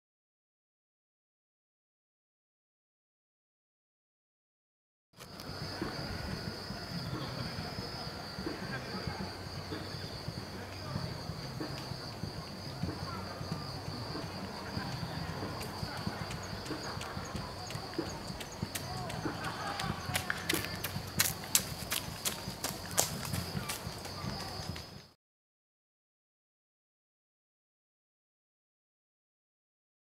Cuckoo snatch a hairy caterpillar and frightened by a jogger (Cuculus sp) HD
at Akigase Park Saitama